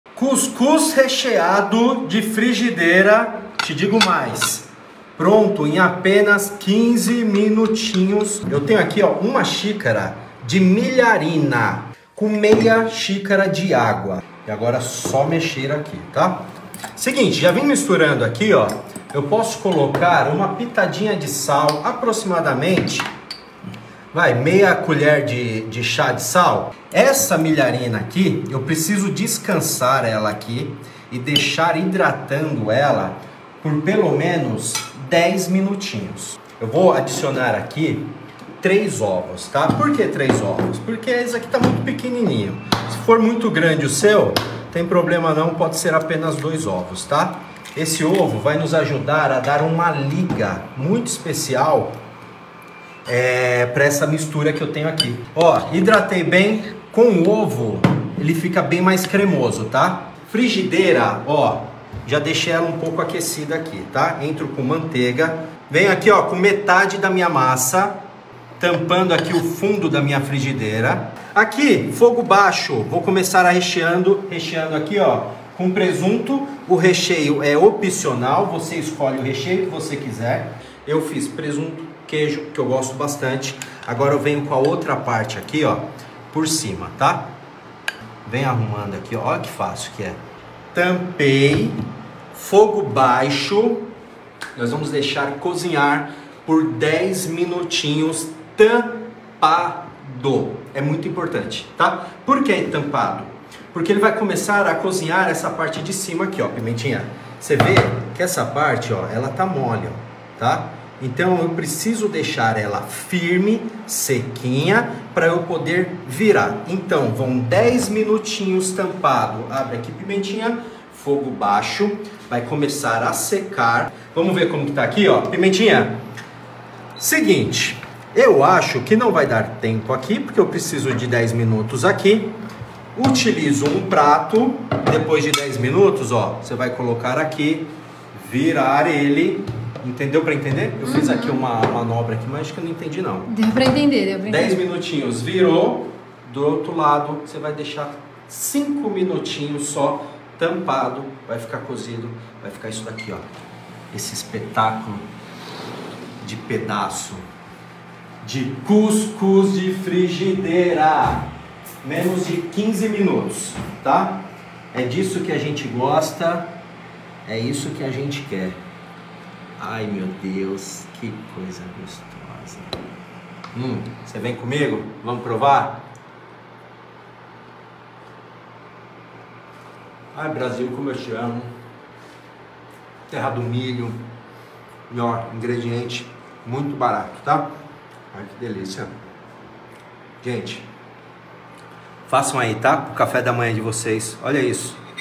Fiz cuscuz recheado para a família toda na frigideira em 15 minutos!
INGREDIENTES:
- 1 xíc. de chá de milharina
- 1/2 xíc. de chá de água quente
- 3 ovos
- manteiga
- sal a gosto
- presunto
- queijo

MODO DE PREPARO:
Hidrate a milharina com a água, deixe descansar por 10 minutos. Assim que hidratar, adicione os ovos, sal e mexa.

Em uma frigideira aquecida, coloque a manteiga e a metade da mistura de milharina. Recheie com presunto e queijo, despeje o restante da mistura.

Tampe a frigideira e deixe cozinhar por 10 minutos em fogo baixo. Depois vire o cuscuz com a ajuda de um prato e deixe cozinhar por mais 5 minutos. Sirva!

Postamos receitas deliciosas 5 vezes por semana!